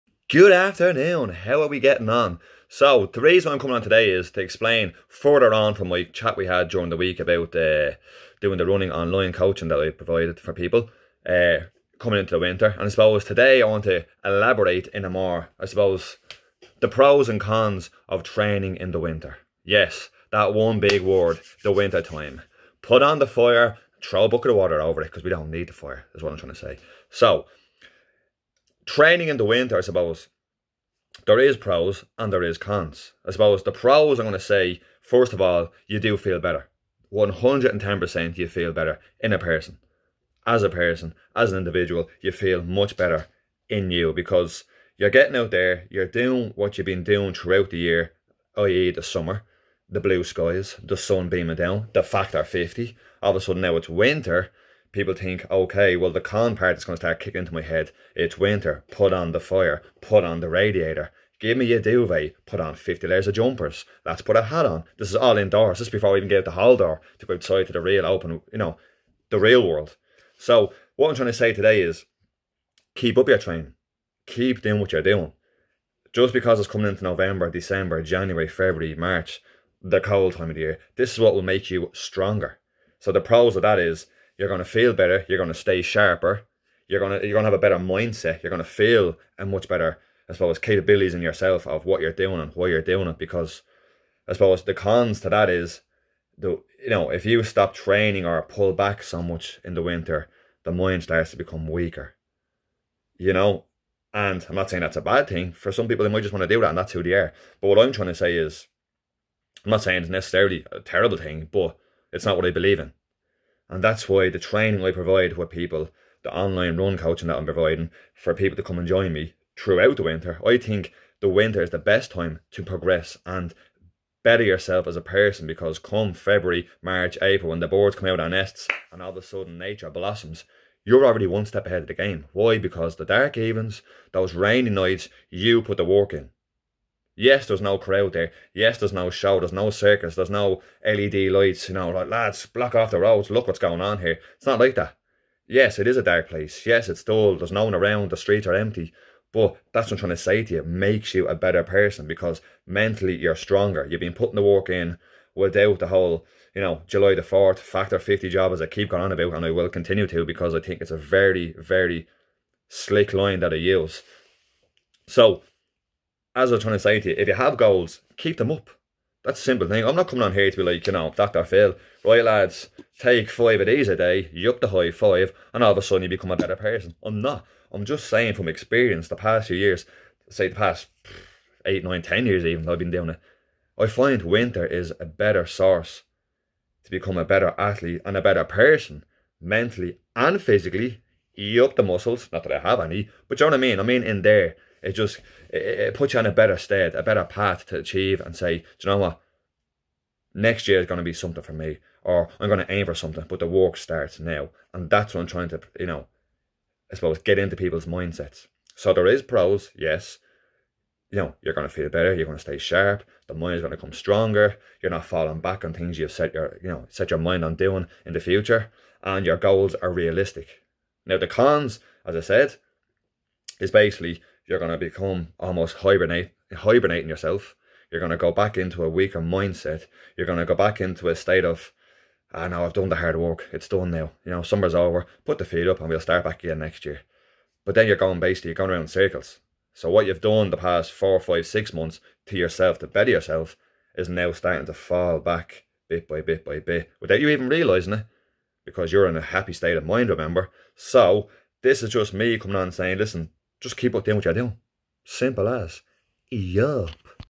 Pro's & Cons's To Training in Winter.
Many have the mindset of exercising in Winter is A no no.

But why should that be the way.

is it because you were brought up that way.
Or everyone else seems to stop so I will to.

This is where one needs to think and do what they want this Winter.